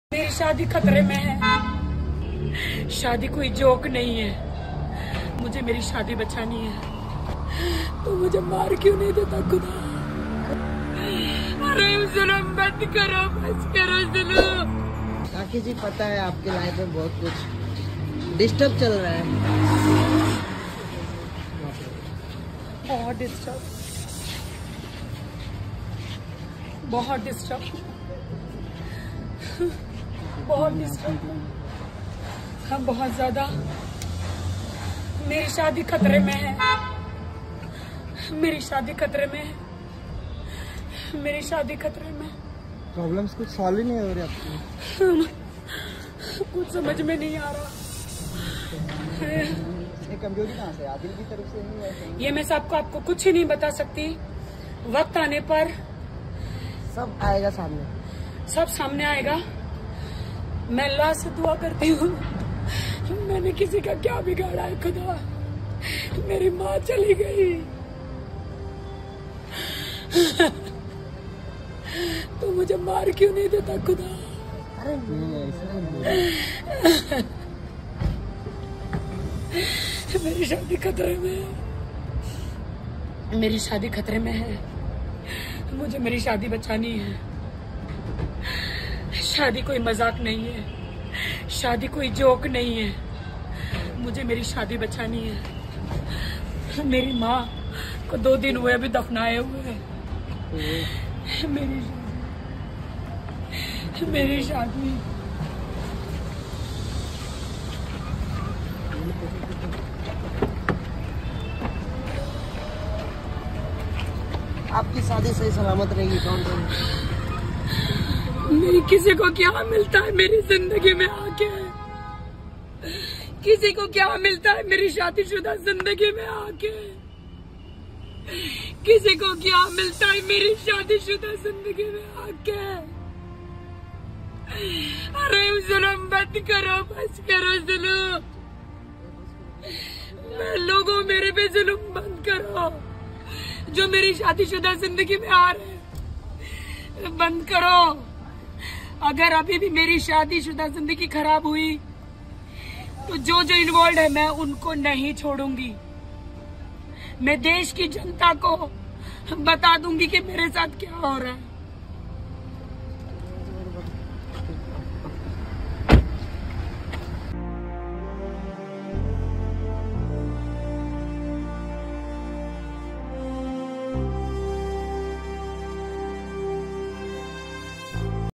तीन और शादियों की धमकी दे रहा Adil Khan, Rakhi ने कहा- मेरी शादी खतरे में...!!
Adil Khan Threatening For Three More Marriages | Rakhi Sawant Adil Khan Durrani Threatening Her For Doing Three More Marriages Rakhi Sawant Says I Do Not Want To Go To The Fridge